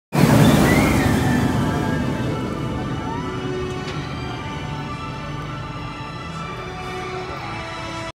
Attraction à Europa Park
Après avoir remporté un concours avec «L'essentiel» et «L'essentiel Radio», 40 personnes ont rejoint Europa Park, à Rust en Allemagne, ce vendredi.